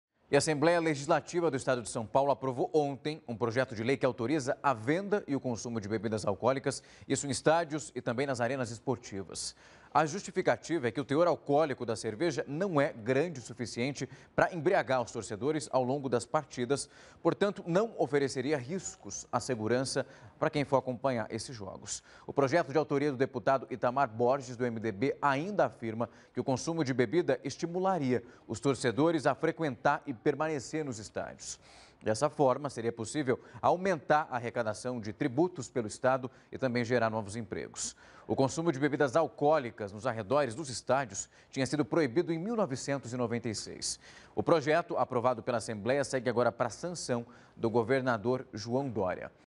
Assembleia aprova venda de bebida alcoólica nos estádios de SP
A Assembleia Legislativa do Estado de São Paulo aprovou nesta quinta-feira (13) o projeto de lei que autoriza a venda e o consumo de bebidas alcoólicas em estádios e arenas esportivas.